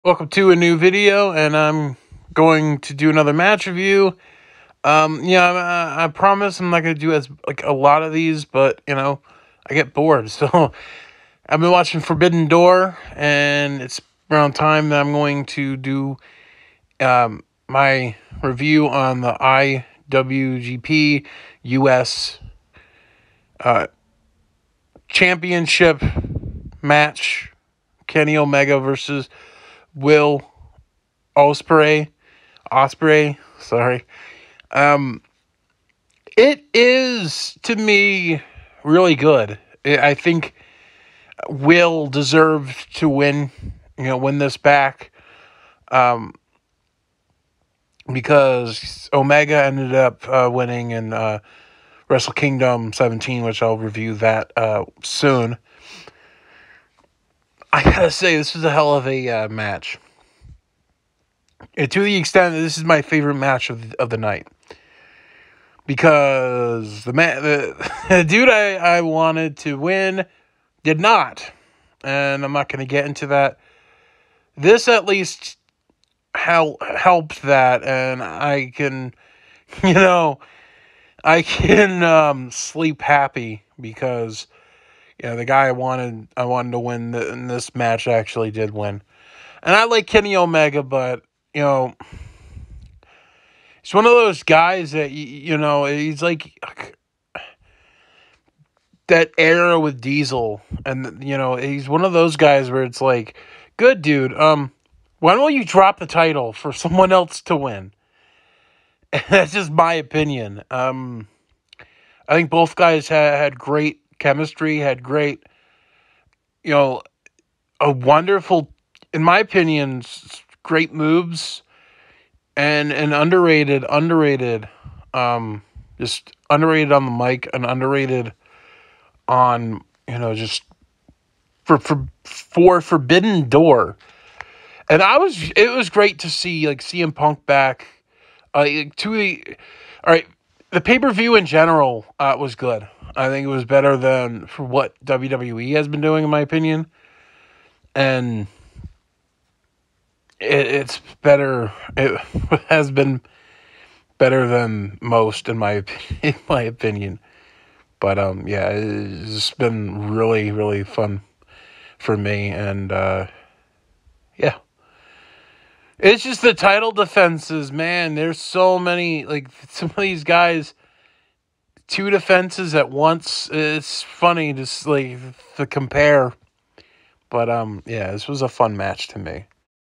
AEW Forbidden Door (2023) - Kenny Omega (C) Vs Will Ospreay For The IWGP US Title Match Review
Last Match Review for a While